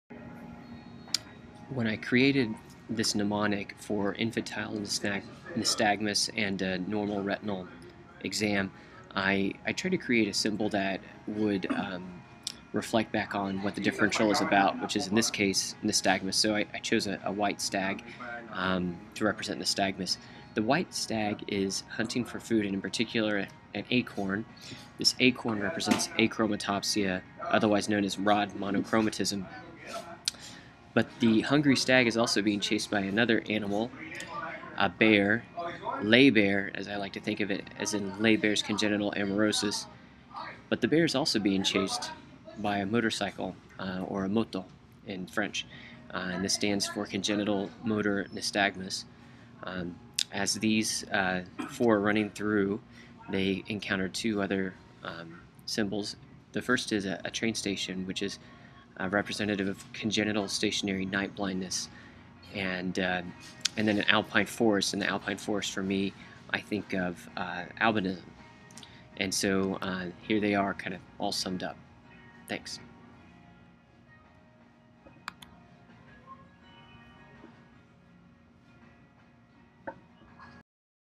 Infantile Nystagmus Ddx Mnemonic
This is a mnemonic device that I use to remember causes of infantile nystagmus in the setting of a normal fundus.